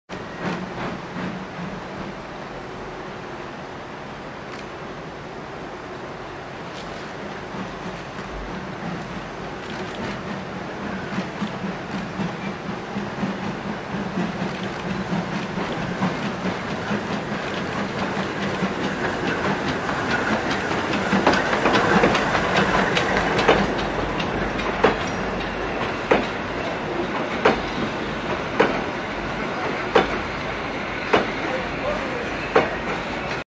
Steam of Tuzla Coal Mines Railways Bosnia(Oct.2015) 25 ボスニア。ツヅラ炭鉱鉄道の蒸気機関車（2015年10月）25
2015年10月撮影。ボスニア・ヘルツェゴビナのツヅラ炭鉱の国鉄線と専用鉄道です。